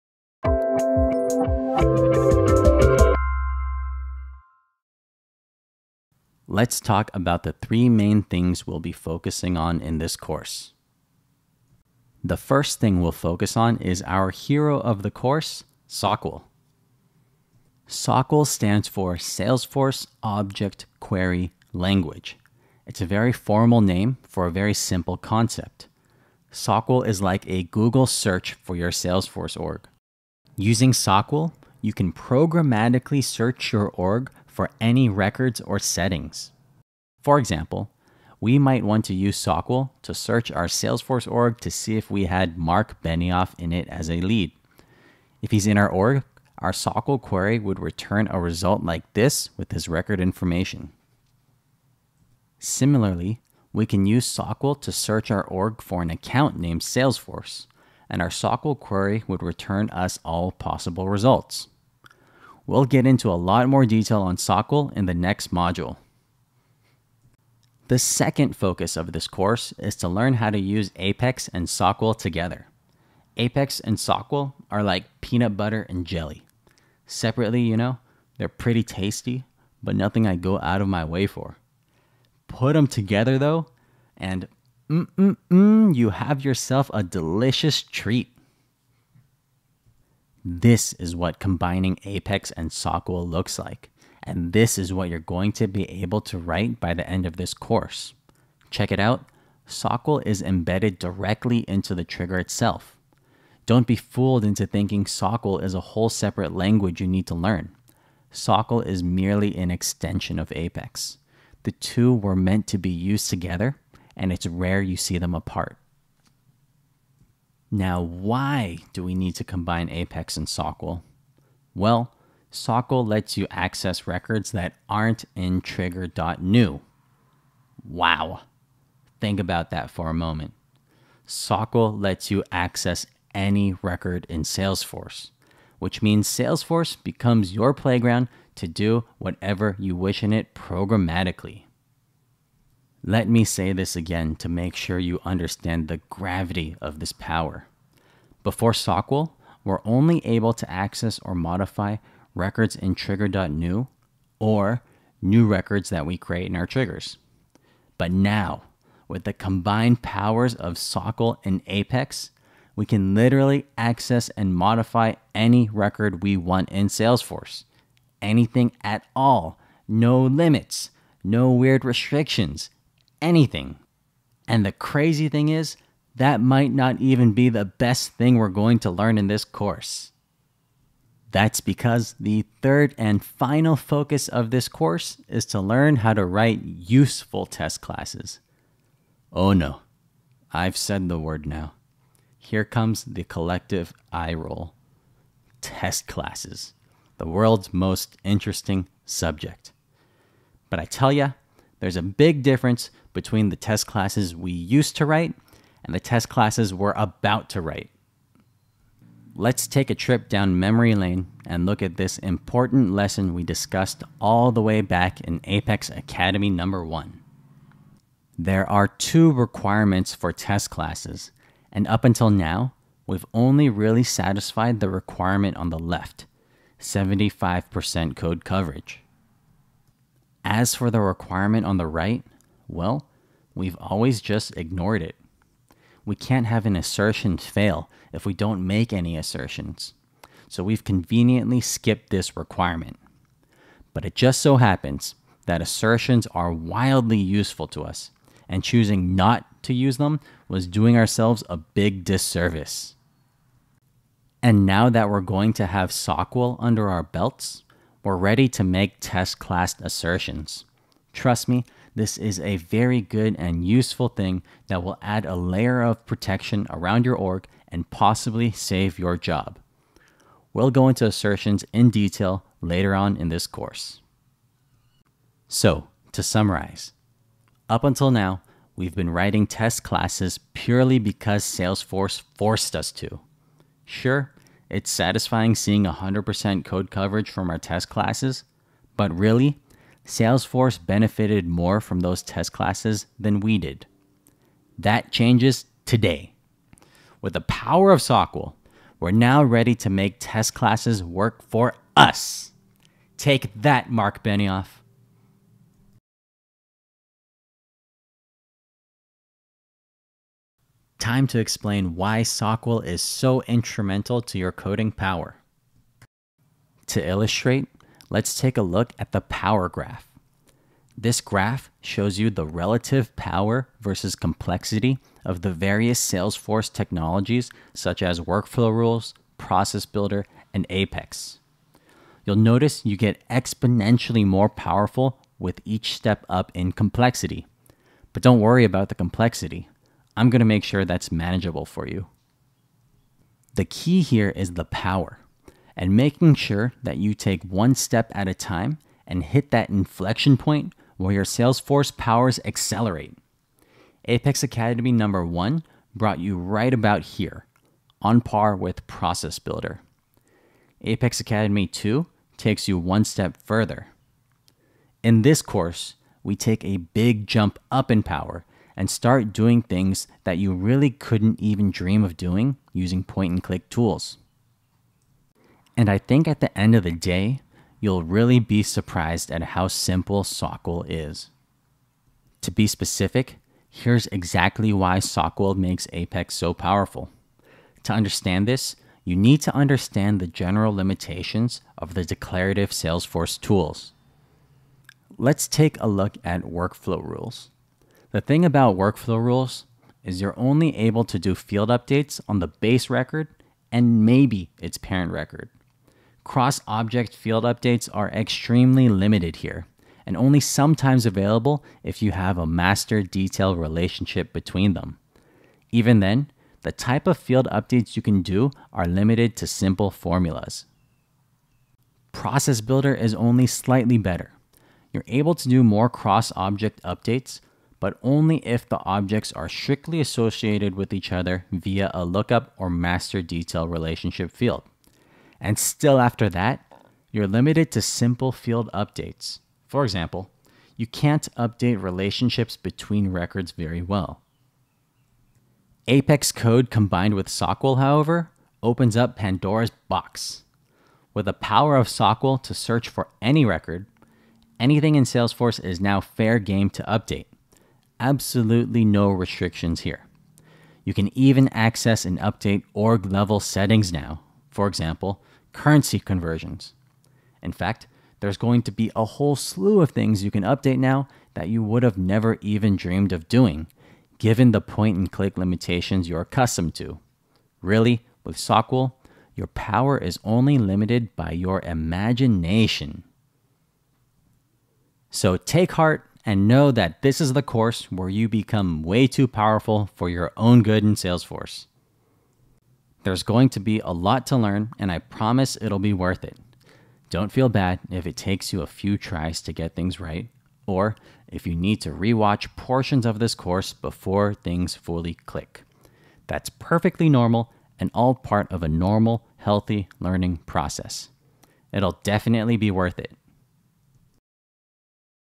Apex Academy: The Power of SOQL
The key to awakening the power of Apex lies in SOQL. Learn how to write SOQL queries, how to integrate SOQL in your Apex triggers, and how to defend your org from bugs by using SOQL in your test classes.